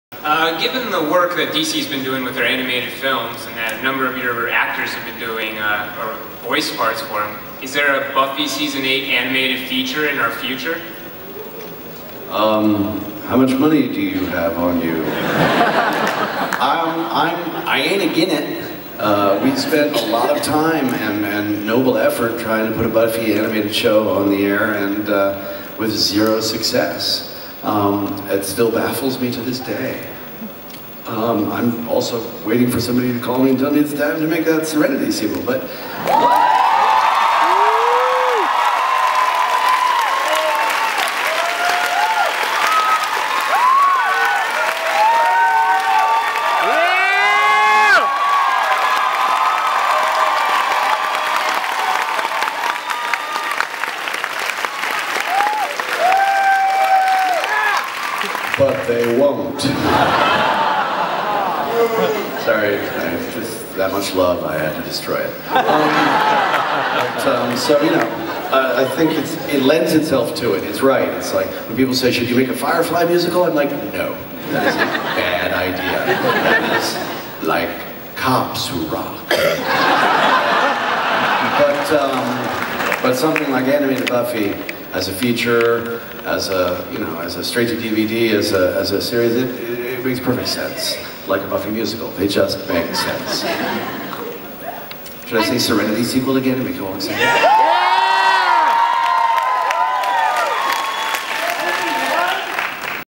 Joss on an animated Buffy and a Serenity movie
Joss Whedon answers a question at the San Diego Comic con asking if he would make an animated Buffy Series.  He also talks about a sequel to Serenity and musicals.